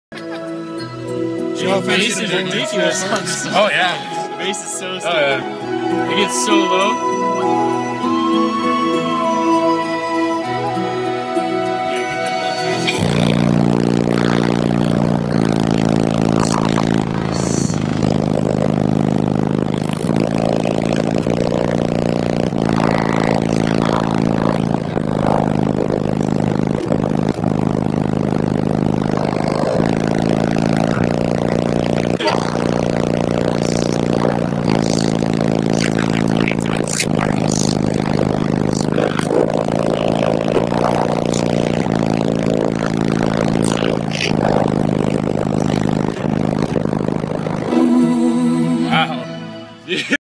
Afrobass hitting me with a clean 24hz!
Hanging out with Afrobass at the Escondido db drag. 2 18" Fi Q's in a wall tuned low powered by a Crescendo BC3500.